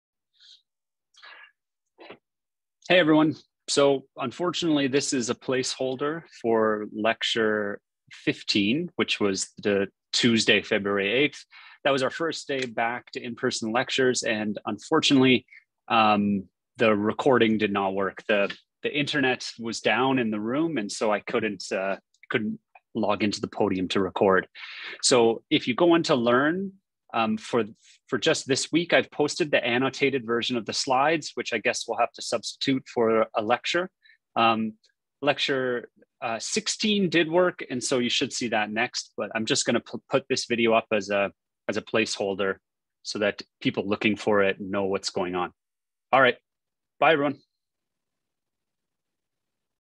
ECE406 Winter 2022 -- Lecture 15: Placeholder, no recording
Unfortunately the internet was down in E7-5353 during this lecture and so I was unable to record the lecture (I need a connection to the podium computer).

We covered up to the end of the proof of correctness of Dijkstra's algorithm (Page 11 of the written notes, page number 118 in the slides).  See the annotated slides on LEARN for this one.